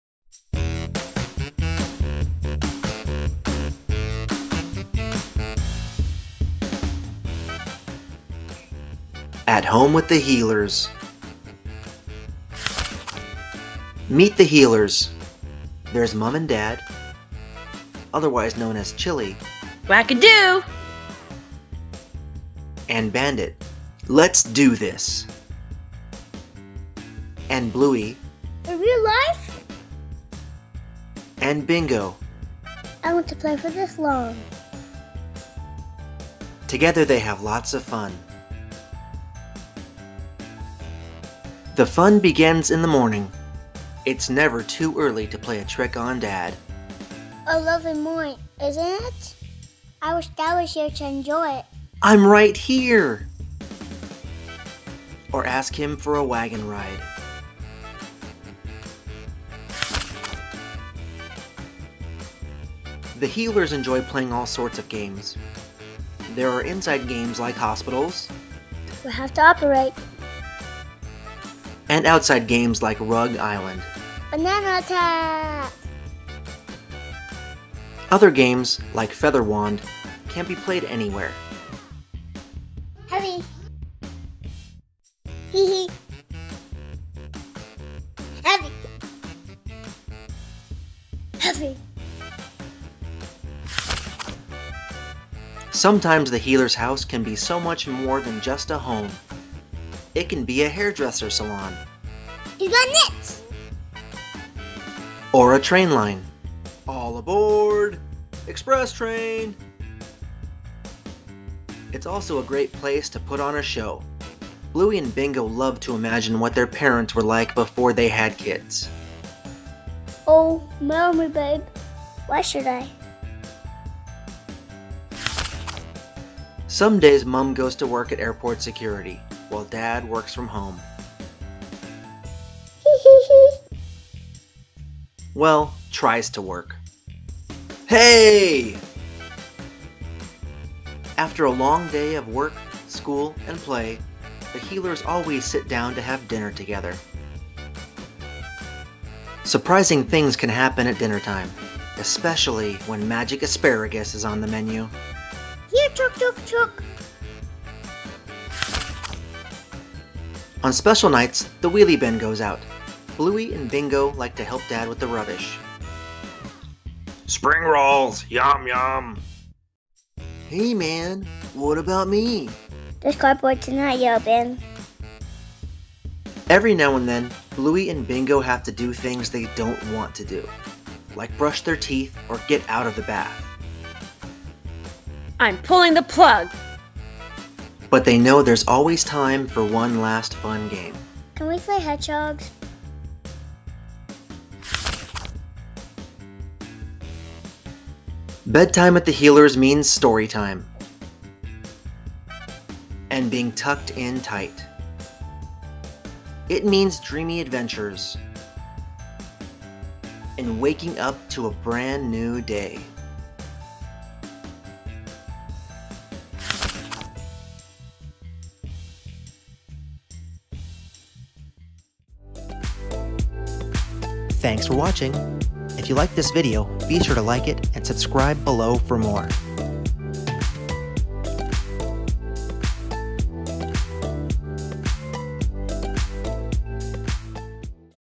Bluey - At Home with the Heelers - Book Read Aloud
At Home with the Heelers - Bluey - Disney Junior Read Aloud
Meet the Heelers - There's Bluey, Bingo, Bandit and Chilli!  Learn all about the family, explore their home, and discover the games they like to play.